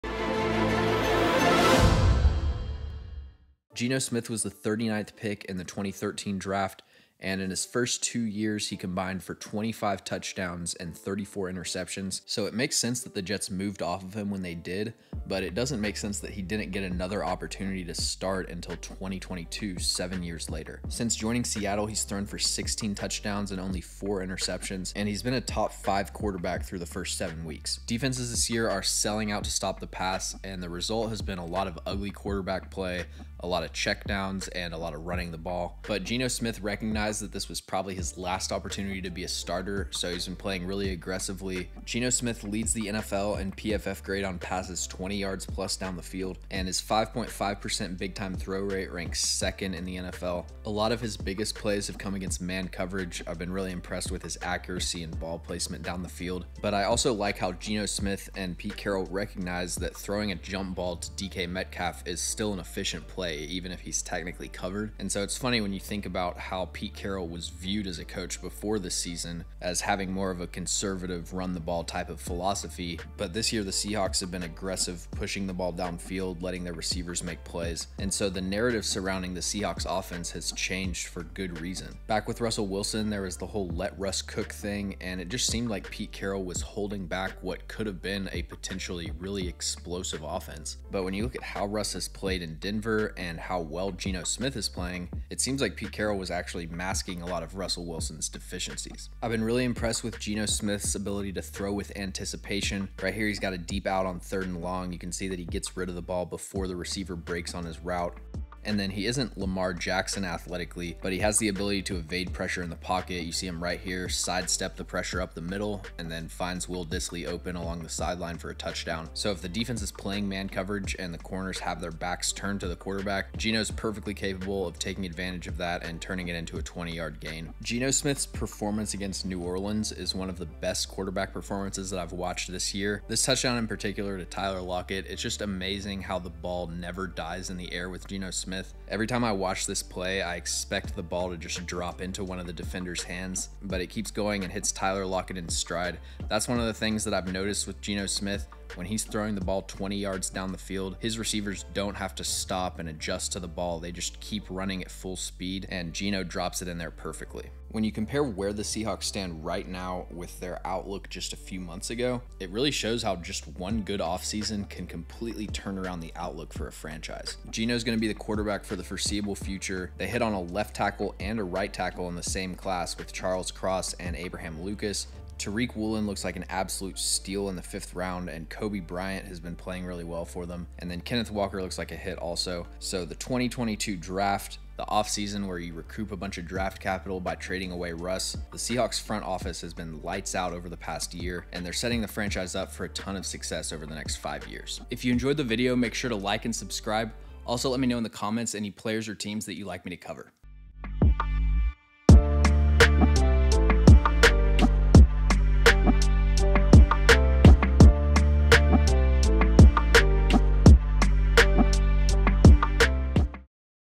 Geno Smith and the Seahawks are COOKING: Film Breakdown
In this film breakdown, we take a look at the Seattle Seahawks and QB Geno Smith, and what is going right for them through 7 games. The Seahawks were expected to take a huge step back after the departure of Russell Wilson, yet Geno Smith has been arguably the best QB in the league through 7 weeks. Here, we break down the Seahawks' offensive scheme, and how it fits perfectly with Geno's style and ability.